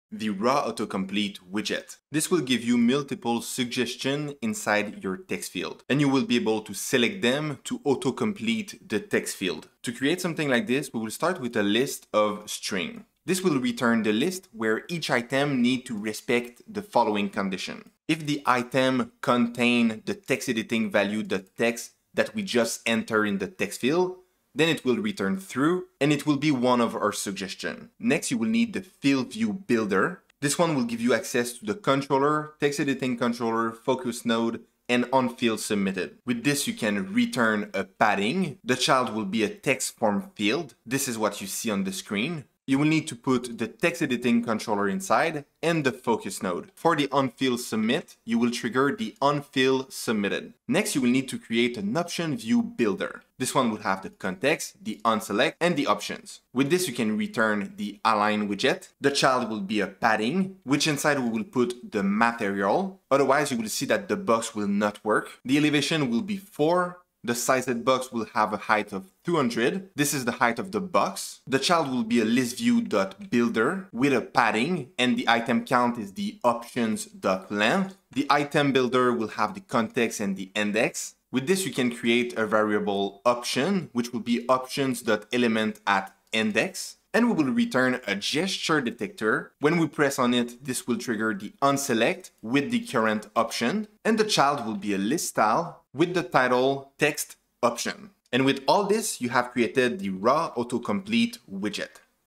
Flutter RawAutoComplete Widget
This Tutorial will show you how to use the RawAutoComplete with flutter. To learn more about every flutter widgets, you can check our flutter playlist about all flutter widgets
This was how to make and use the RawAutoComplete widget within your Flutter app

OVERVIEW
0:00​ RawAutoComplete Flutter